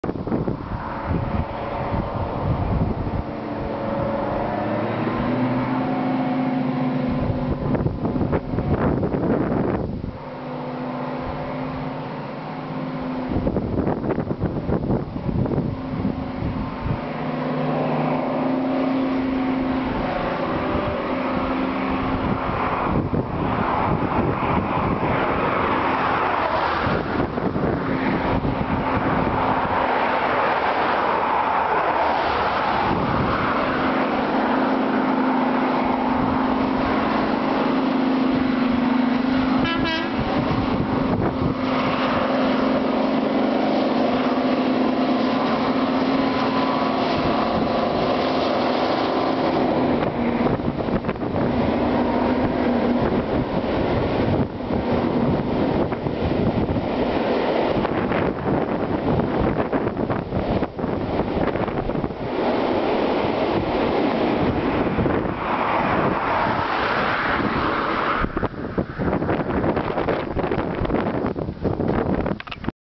55022 Royal Scots Grey - Kirkham 19th July 2008 [Mid Wales Borderer]
Mid Wales Borderer - Blackpool-Cardiff and return.
Seen at Kirkham at 06.40. This is the 3rd [*edit*] EVER production Deltic to run along the Blackpool line between North and Preston